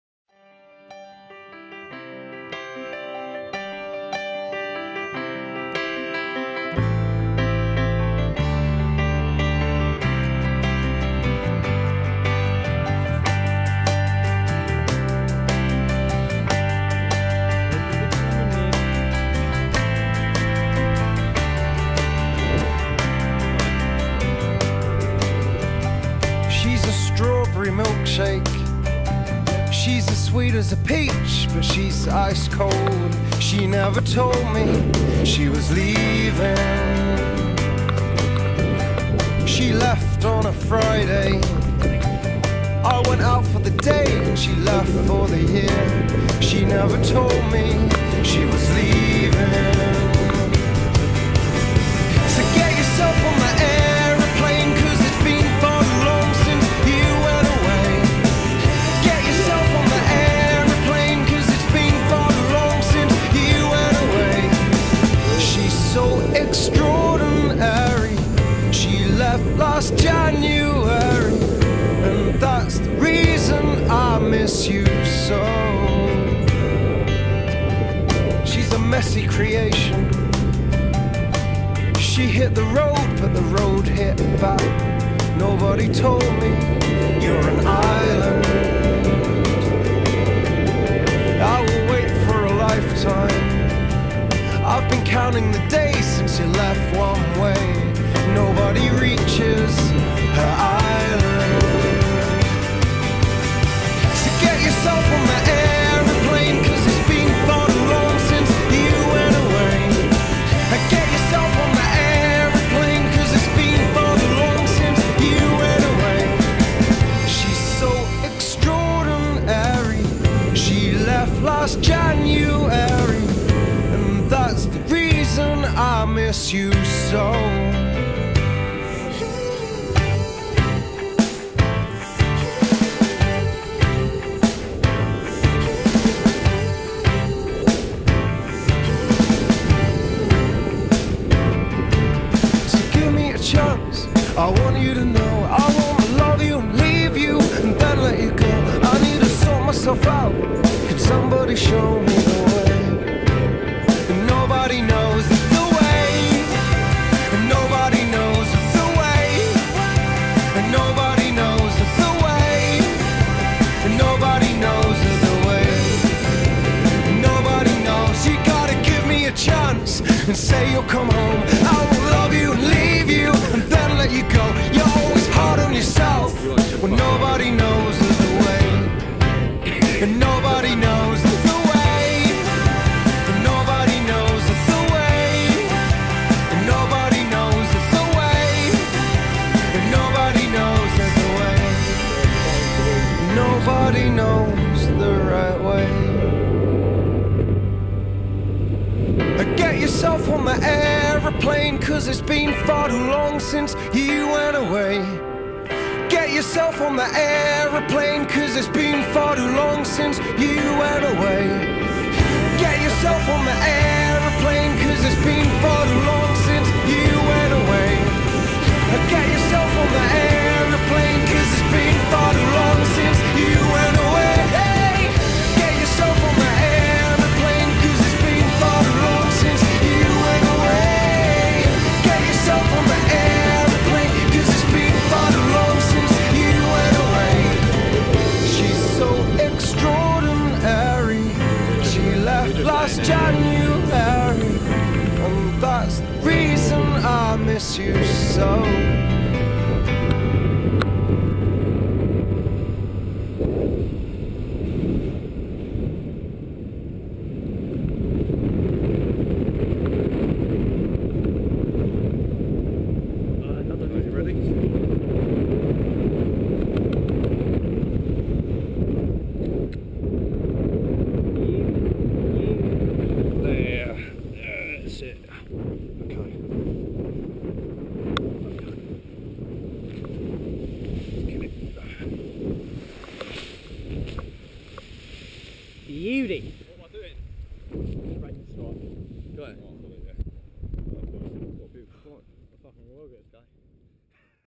first tandem movie
My first Tandem flight in Dune Du Pyla France